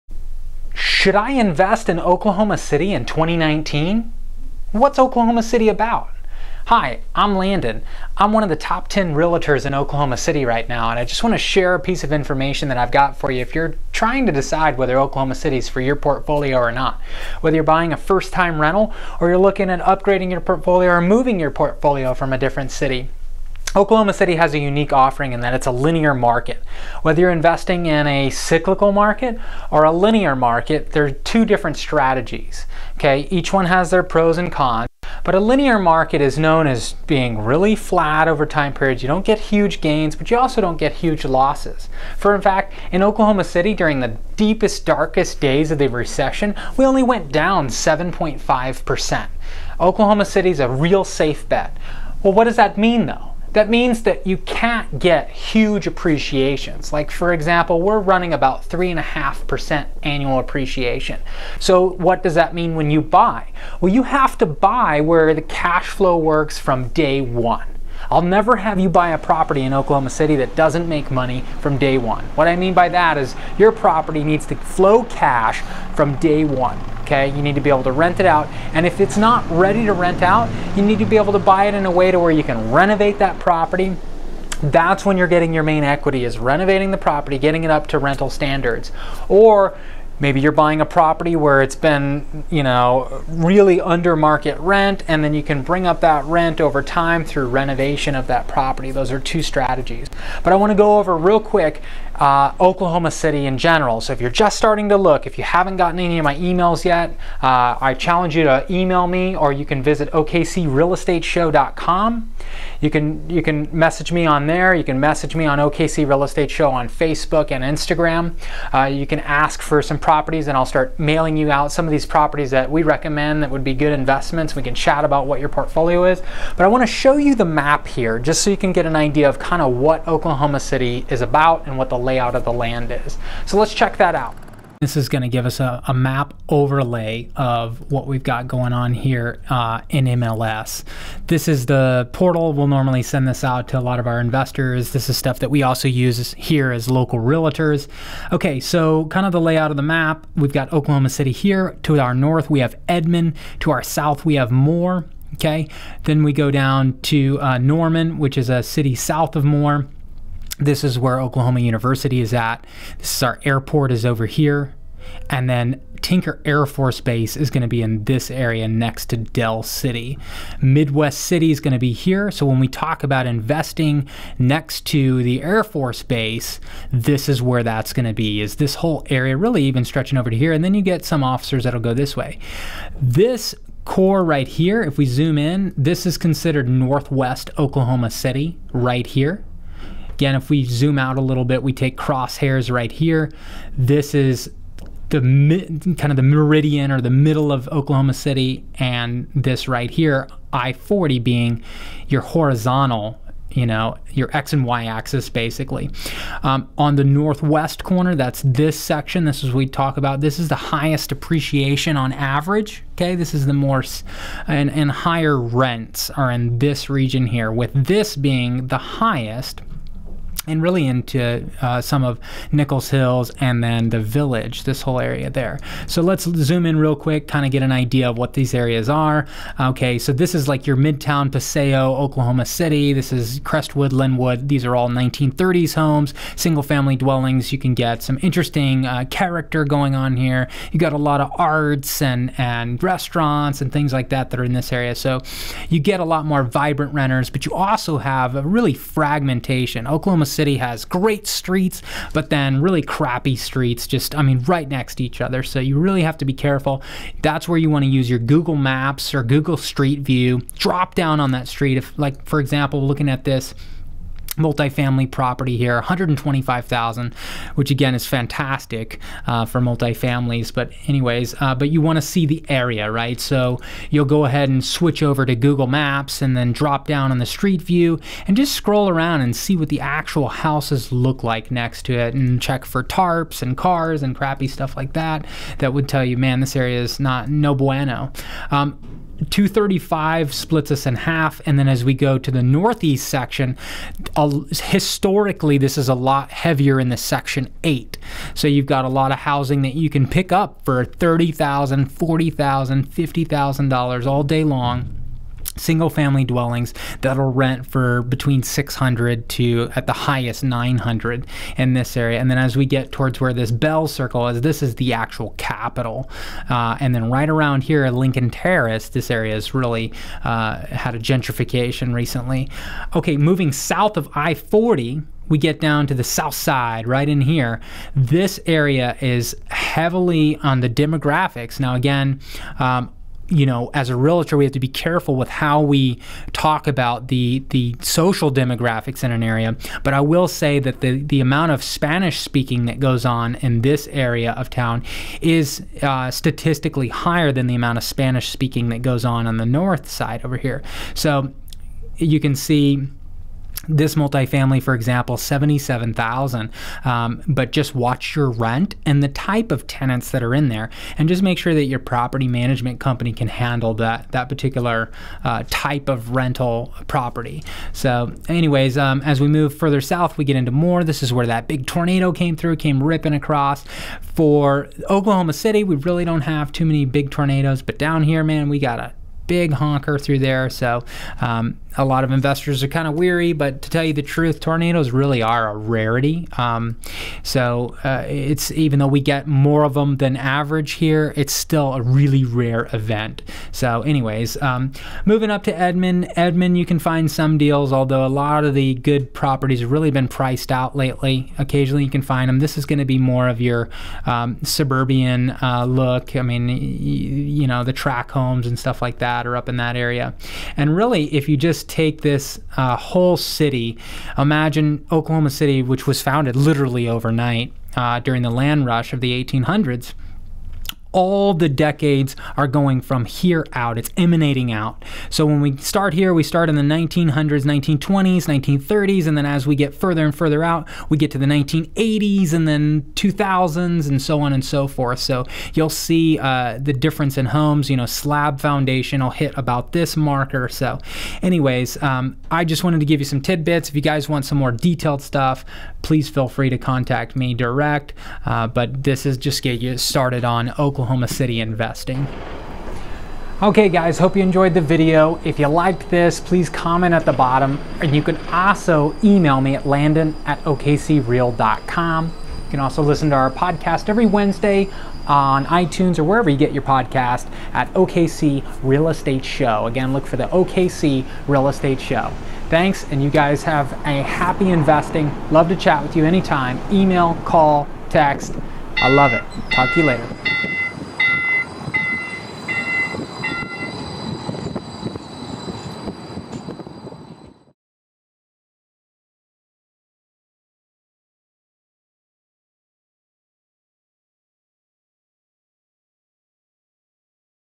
Oklahoma City Real Estate Investing 2019
Where and how to invest in Oklahoma City in 2019.

Read over 40+ reviews on other investors experience with me